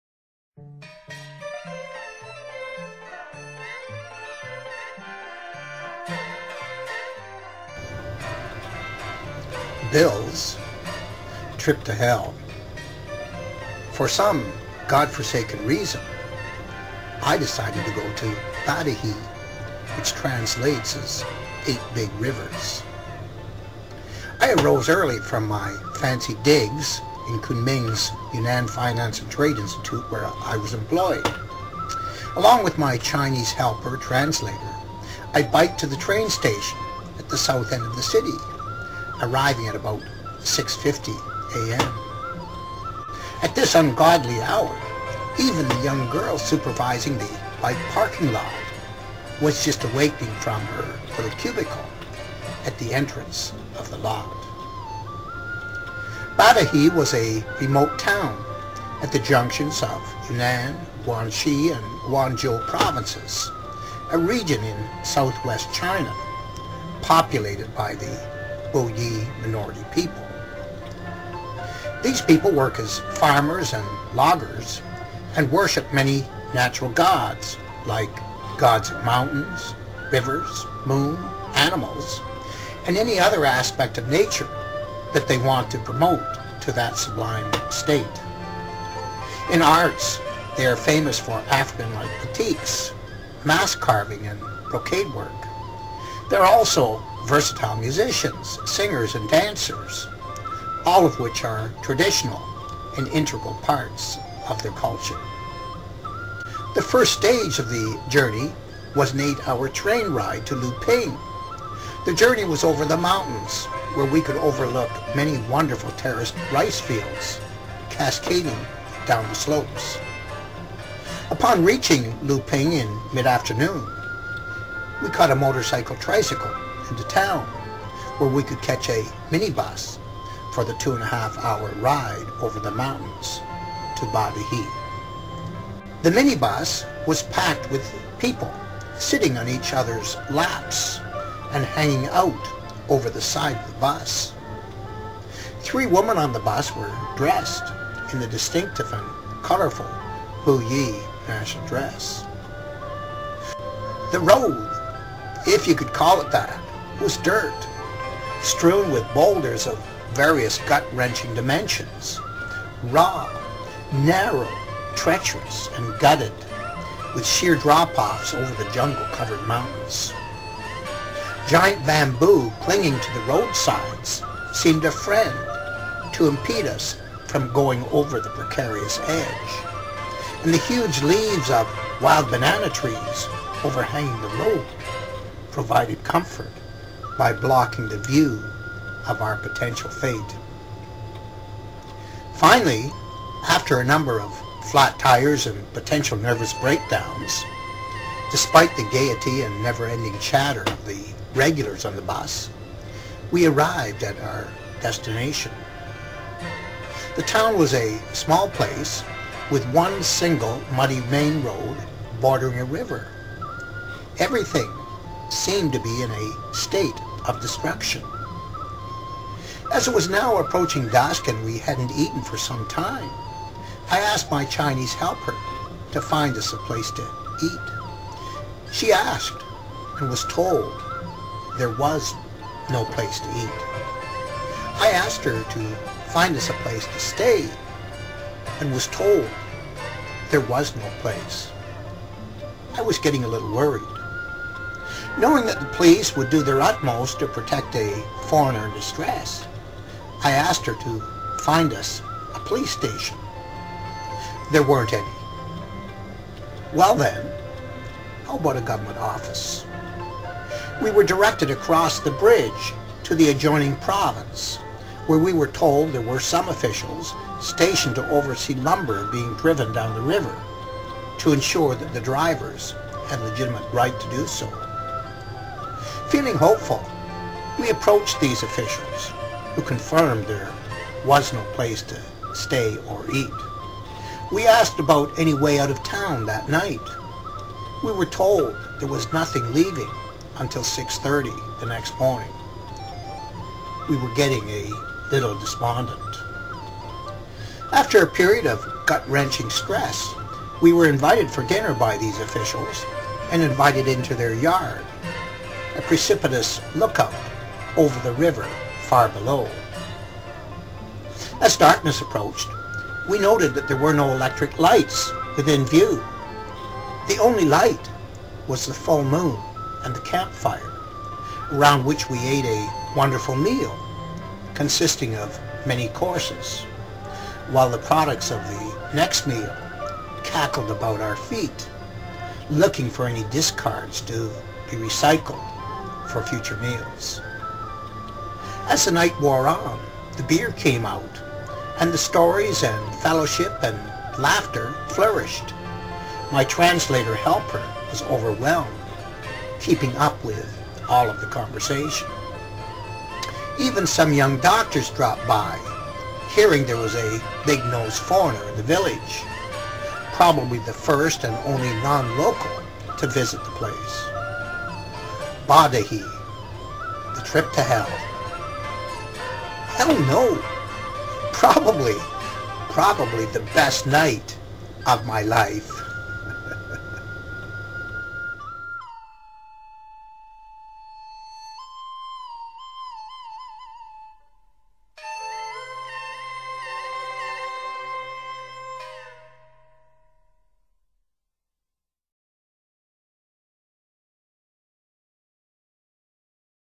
The Trip to Hell
Bill Longworth recounts a trip to an out-of-the-way place in SW China in this video that trapped him in a dangerous place where he shouldn't have been with no way to escape, no place to stay, and no place to eat.